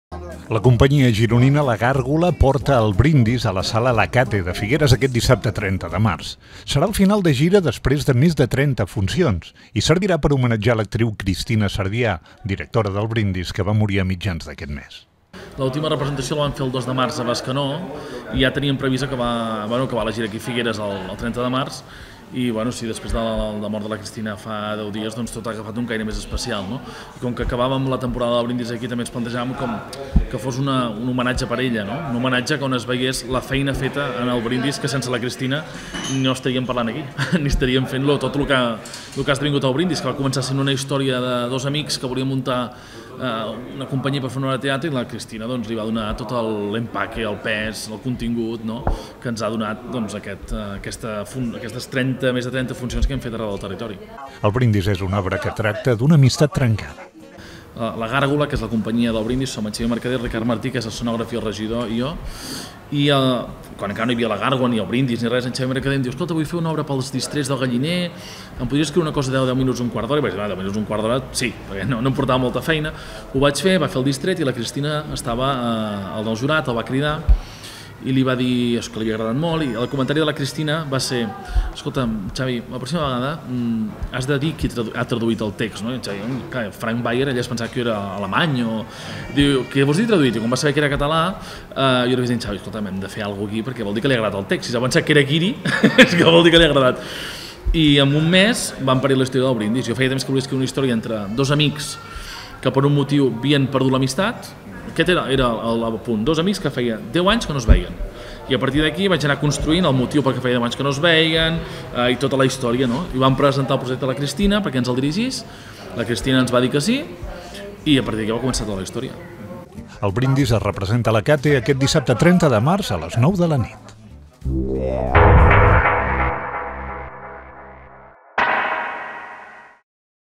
"El brindis" de La Gàrgola Produccions tanca la seva gira a Figueres
El dramaturg Frank Bayer ens parla de l'obra "El Brindis" de La Gàrgola Produccions que arriba aquest dissabte a La Cate de Figueres (21:00).